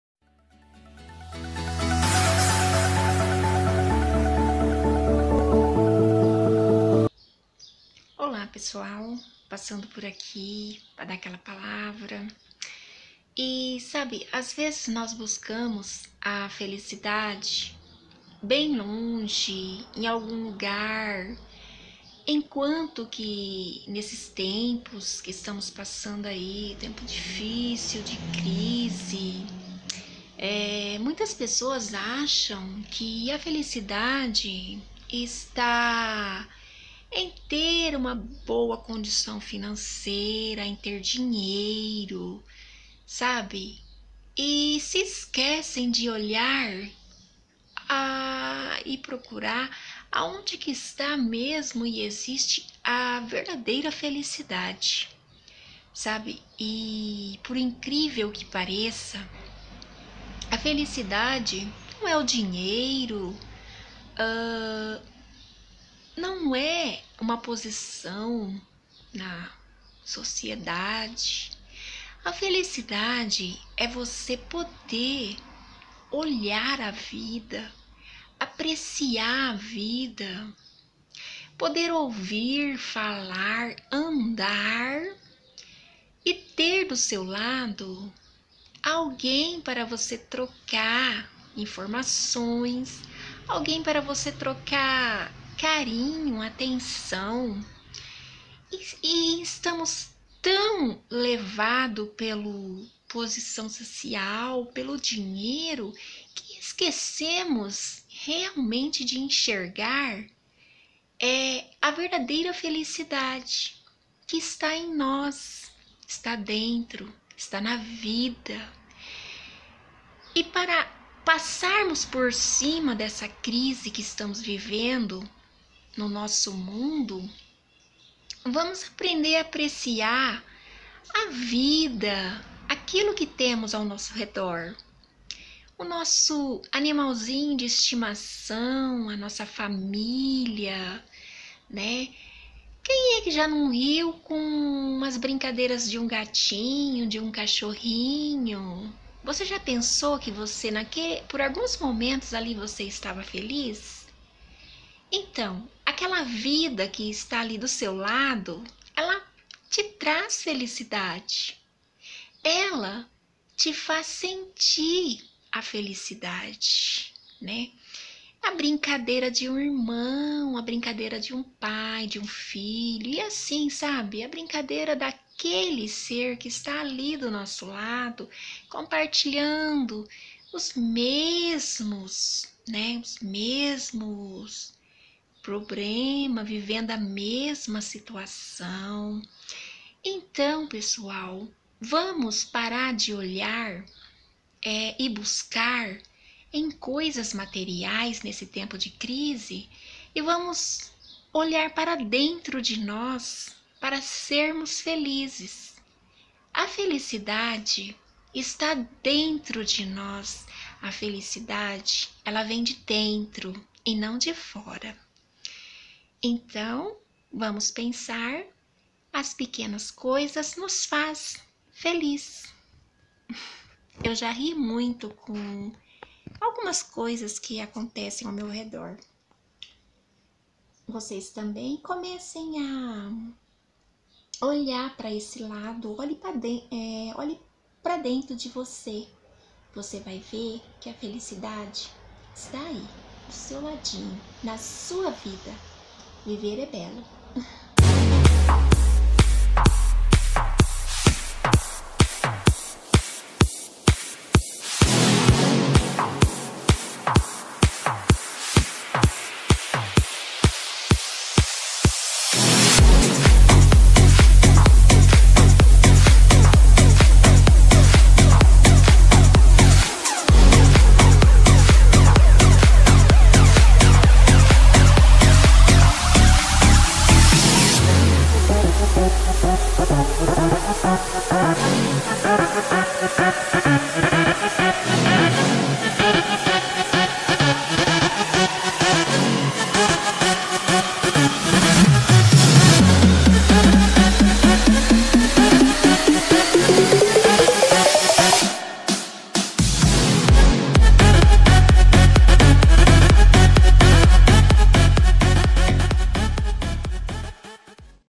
A felicidade esta em nós
não deixe nada te derrubar aprenda a amar tudo a sua volta que seus problemas diminuem

FE E FOCO